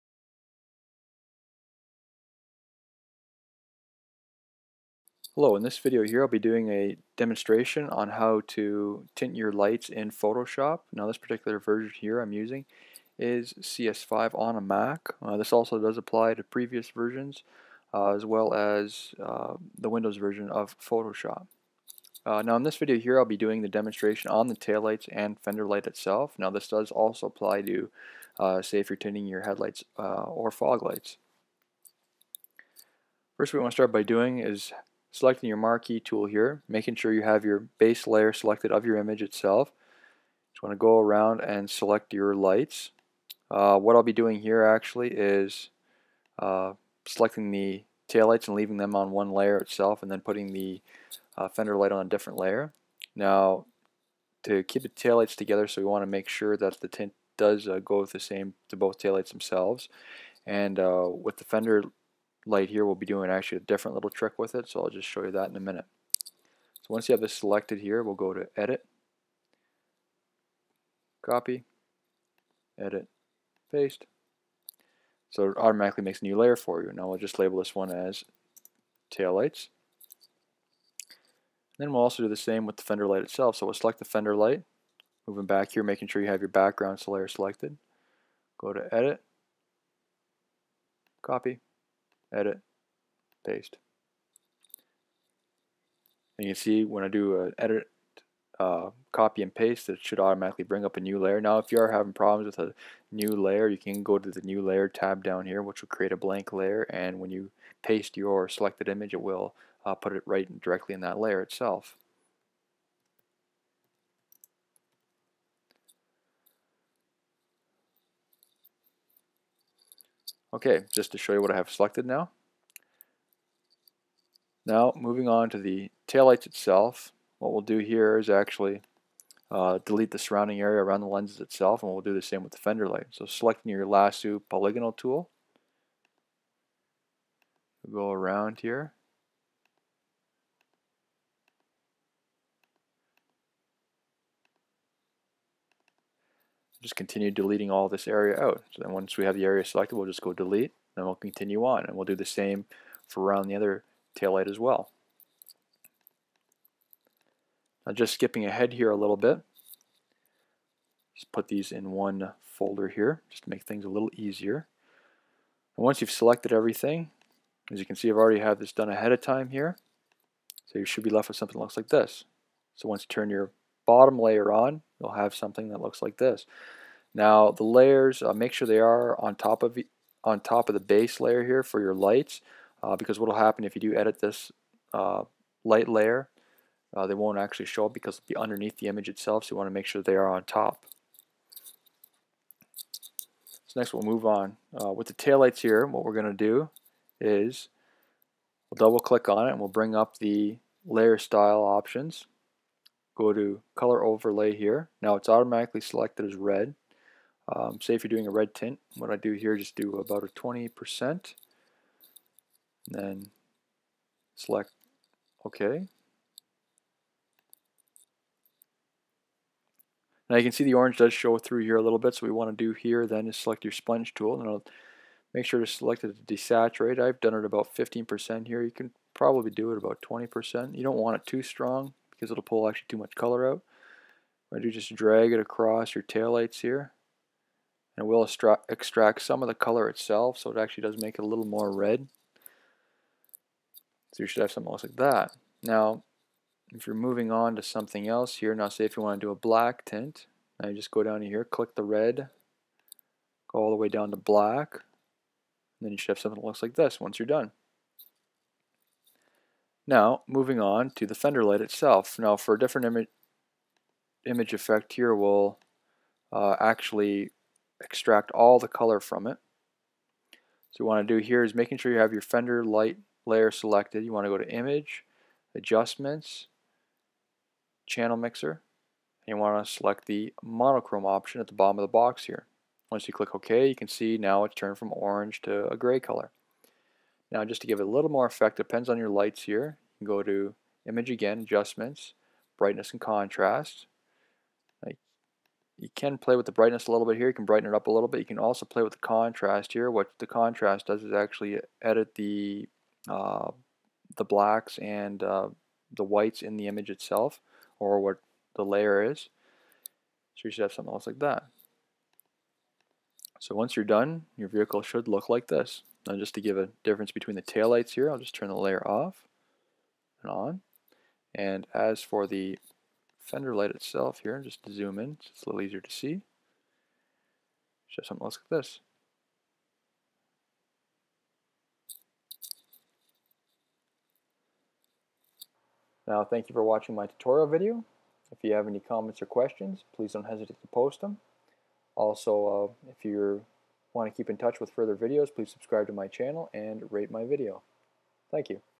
Adobe Photoshop: Tinting Lights on a Vehicle
Video tutorial on how to tint the lights on a vehicle in Adobe Photoshop. This was done on Adobe Photoshop version CS5 on a Mac, but this also applies to other versions of Adobe Photoshop and on the Windows version as well.

Tools/Supplies Needed:
-Adobe Photoshop
-computer
-image

Procedure:
-first start by picking a photo you want to tint the lights of the vehicle
-using the marque tool, select a rough outline for lights which you want to tint
-once you have the desired area, go to edit/copy and then edit/paste
-a new layer should automatically appear with the selected area for the lights
-now this step is optional, but does make the process a little easier and less confusing
-label the lower layer as "base" and the above layer as "tail lights"
-make the "base" layer invisible, then selected the "polygonal lasso" tool
-make sure you have the "tail lights" layer selected, using the "polygonal lasso" tool to select the non glass area and delete
-turn on the base layer to visible
-now double click on the "tail light" layer or left click and select "blending style" on the pop up menu
-go to color "color overlay", make sure the box is checked, and select what color you would like to use for tinting
-edit the opacity to what ever your desired darkness is
-I chose 20% in this tutorial video
-if you have a color such as orange on the light, select the "sponge tool", desaturate to about 15%
-drag the tool across the unwanted coloured area
-to remove color from an object, go to image/adjustments/channel mixture and check the monochrome box
-to adjust the tones of the object go to image/adjustments/brightness and contract and alter the brightness and contrast to your liking
-once you are satisfied with your image, you are ready save and you're done